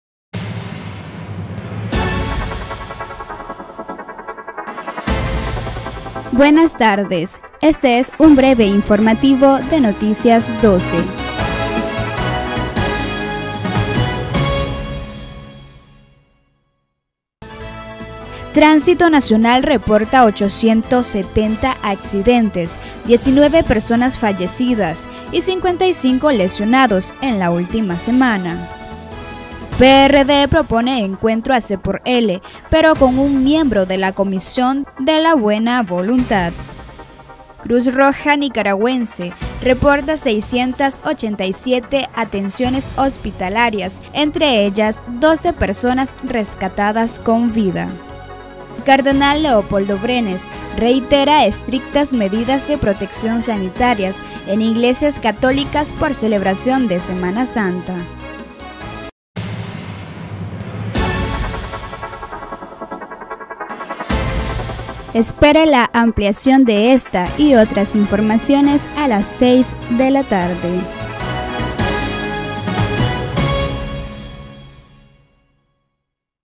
Breve Informativo de Noticias12
📍Tránsito Nacional reporta 870 accidentes de tránsito en la última semana
📍PRD propone encuentro a CxL pero con un miembro de la Comisión de Buena Voluntad.
📍CRN reporta 687 atenciones hospitalarias en inicio de Semana Santa.
📍Cardenal Brenes reitera estrictas medidas de protección sanitarias en iglesias católicas.